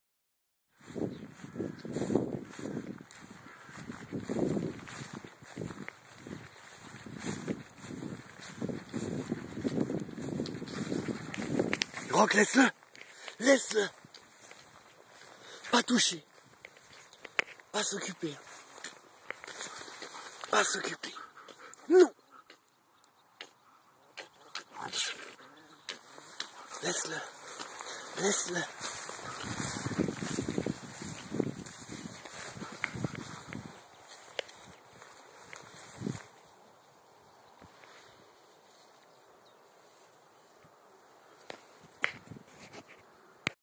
chien vs renard
Rencontre d'un renardeau avec mon chien Urock sur l'île de montigny prêt de la voie verte a Vireux-wallerand le 16 mai 2012